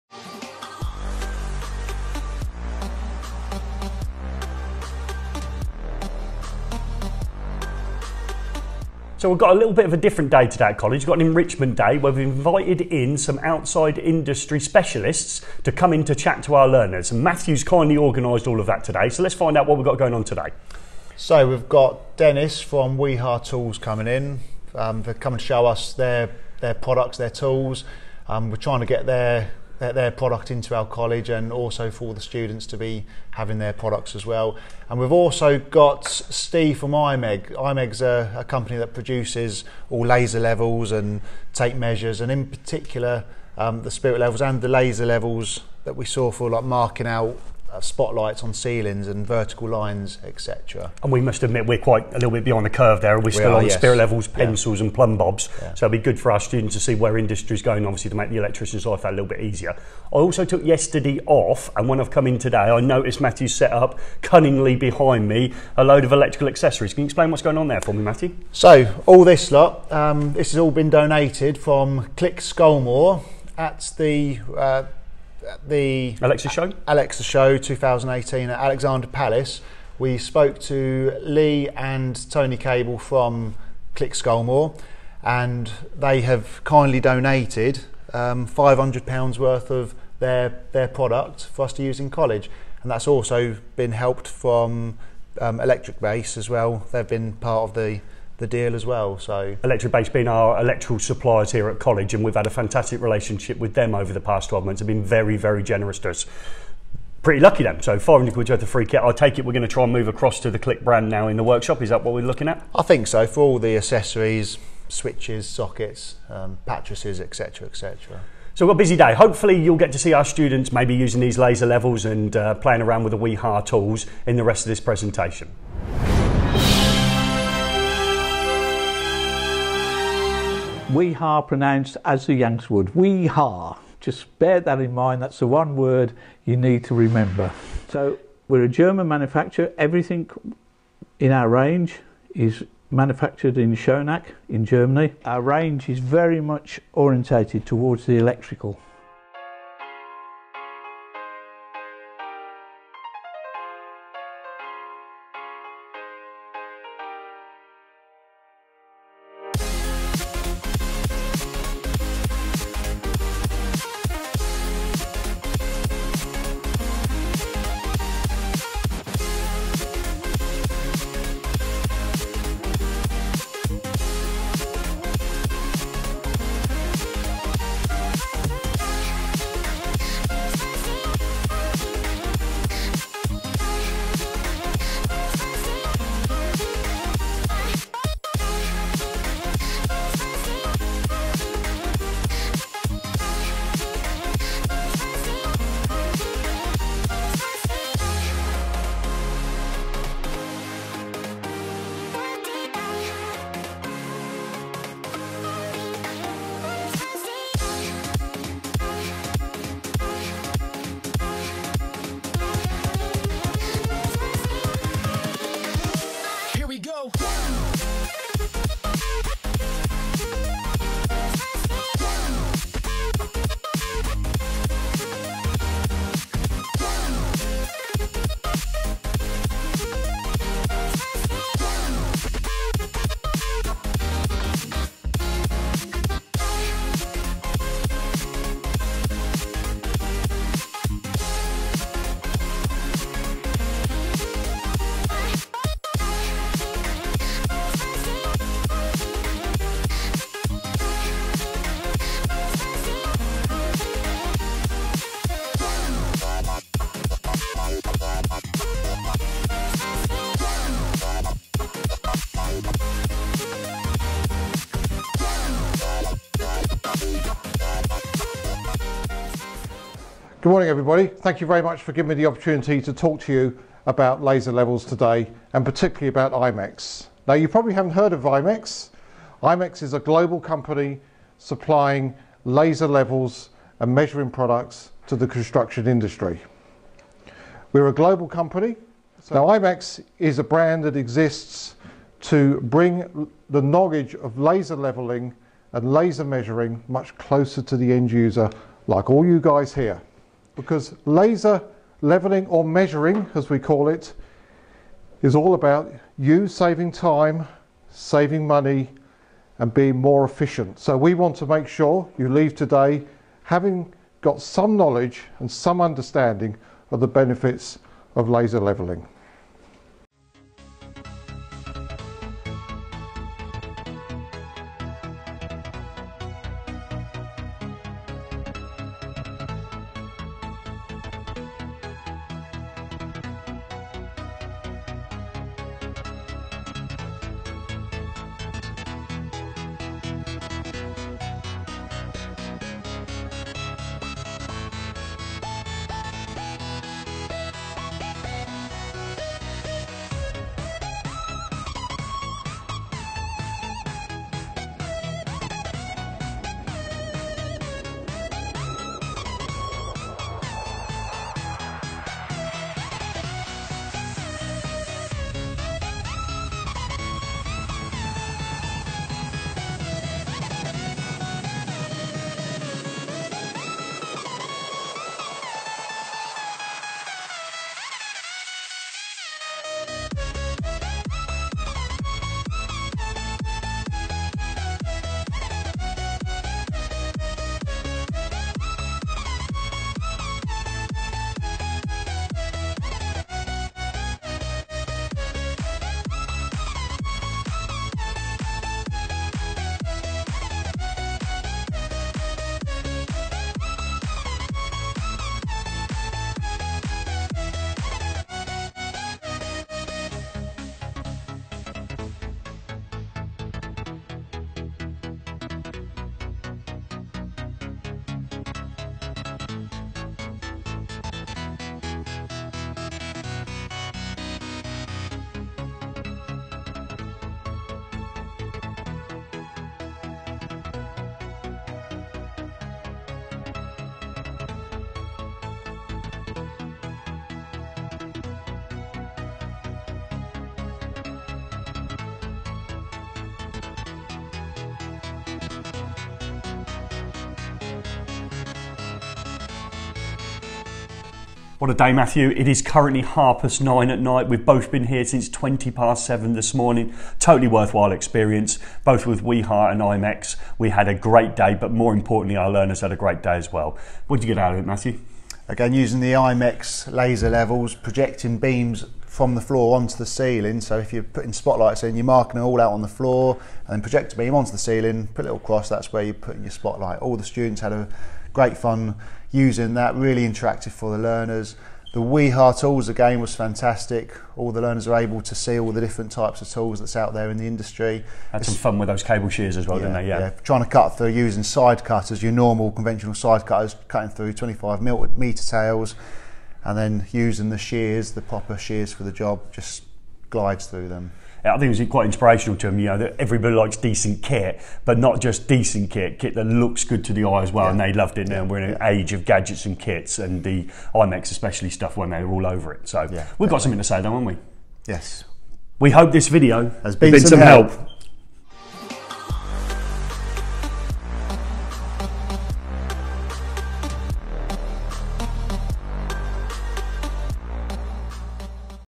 Enrichment Day for the Electrical Students Includes imex Laser Levels and Wiha Tools
It's not all work and no play for the learners at Tresham College as we invite Wiha tools and imex laser levels in for the day. We greatly appreciate the time they spent with us one that was positive for all learners.